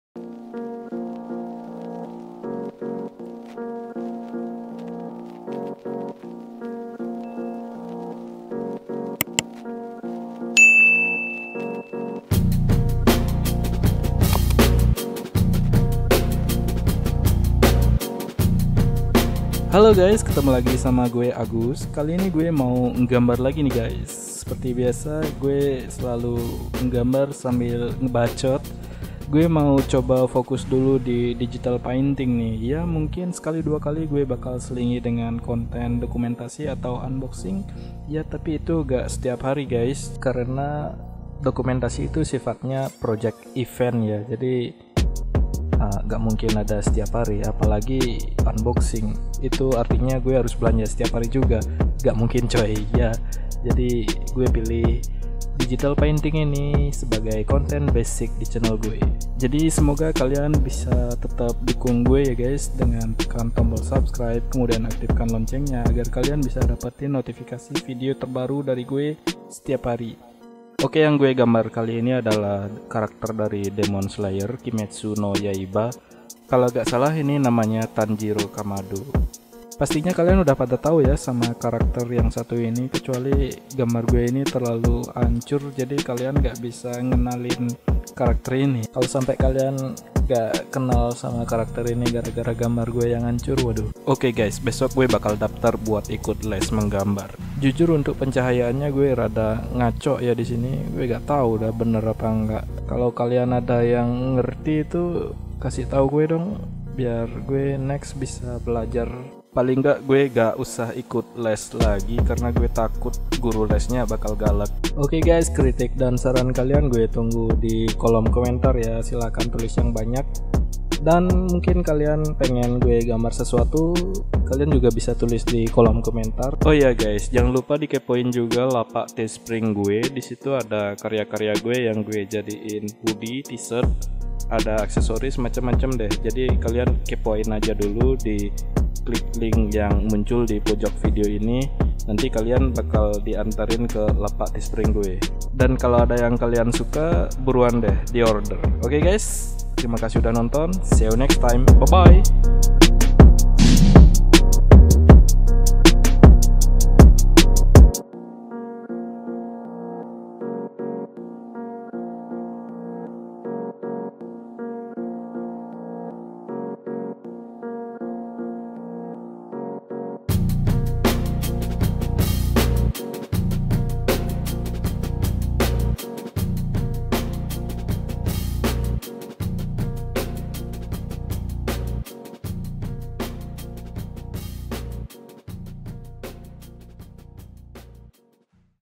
Digital Painting - Demon Slayer Tanjiro
Halo Guys apa kabar kalian semua,
kali ini gue bikin video digital painting
Demon Slayer Tanjiro kamado
maaf ya guys kalo masih kurang sempurna
gue masih baru belajar wkwkwkwk.,,
semoga kalian suka ya guys,,
dengan Digital Painting - Demon Slayer ini
di sini gue gambar pake ipad 6 2018,
app adobe fresco dengan apple pencil
generasi pertama. hasil gambar ini
gue pos di instagram dan
web tesspring gue silahkan di kepoin dulu guys
cek ling di bawah sini OK
thx udah nonton video Digital Painting - Demon Slayer